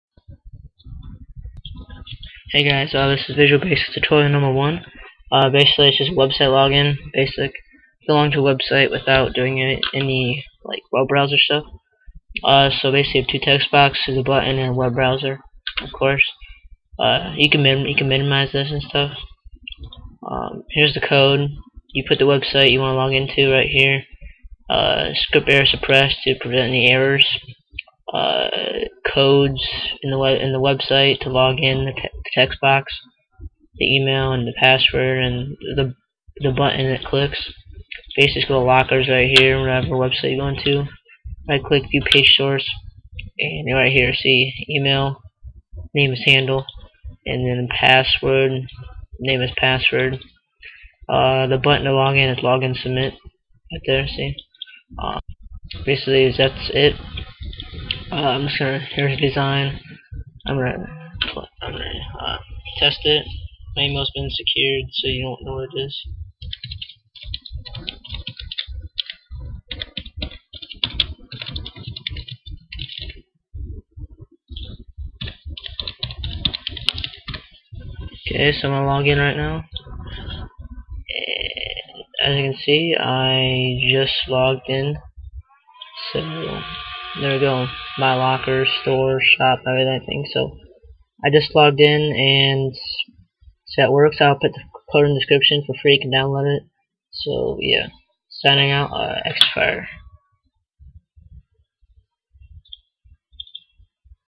{VB.NET}∞Tutorial 1 - Website Login
Hi, Codes and Project Today I will be showing you how to login to a website with a only a form in Microsoft Visual Basics 2010 or 2008